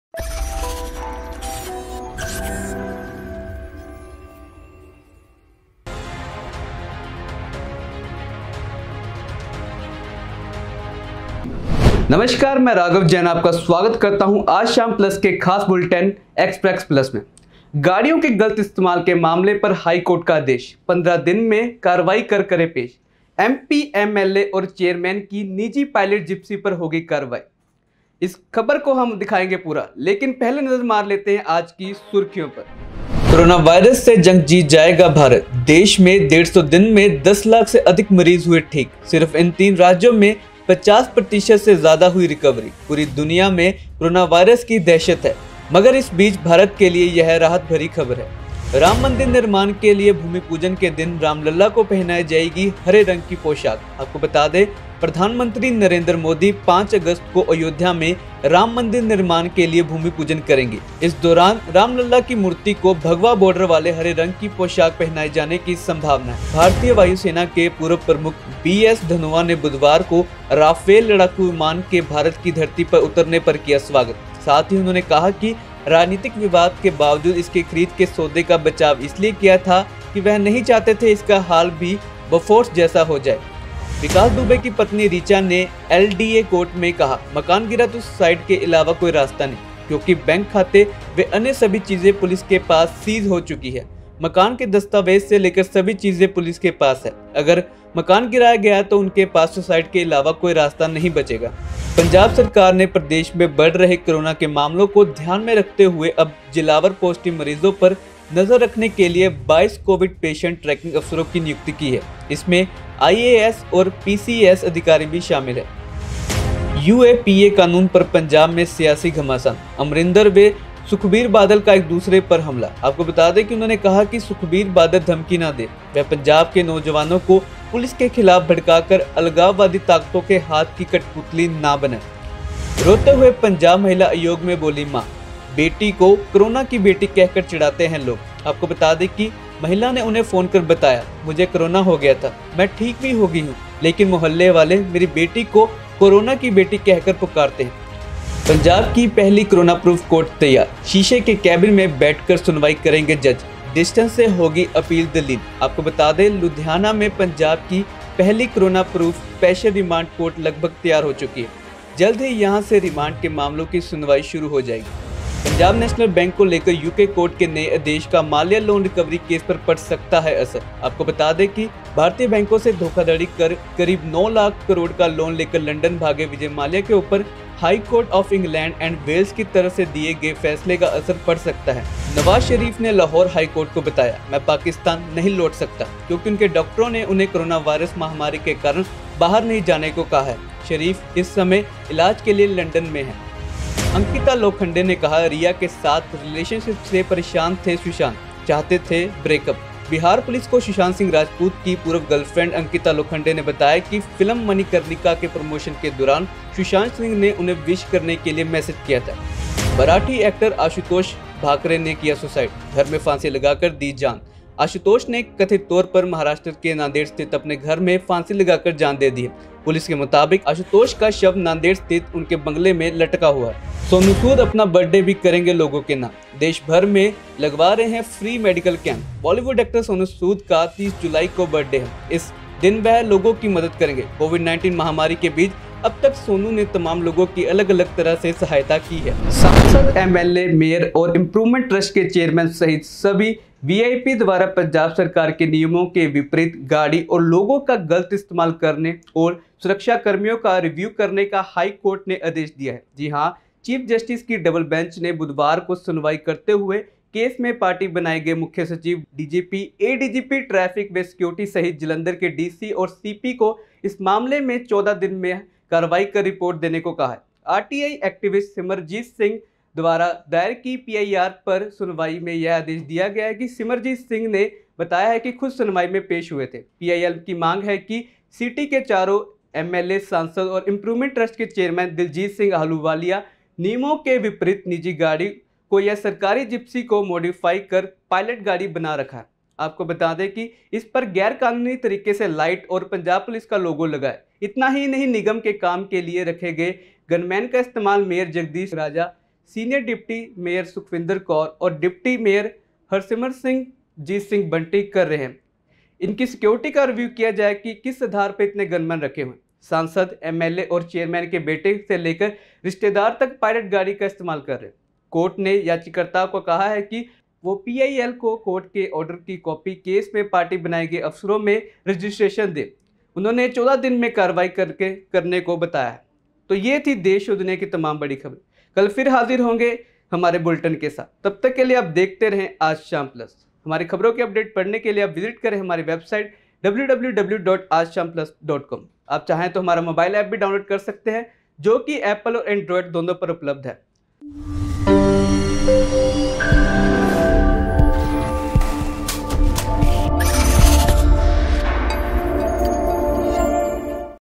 Express plus| स्टेटस बढ़ाने के लिए नेता कर रहे गनमैन और गाड़ियों का दुरुपयोग| raghav jain
गाड़ियाें के गलत इस्तेमाल के मामले में हाईकोर्ट का आदेश- 15 दिन में कार्रवाई कर पेश करें
एमपी, एमएलए और चेयरमैन की निजी पायलट जिप्सी पर हाेगी कार्रवाई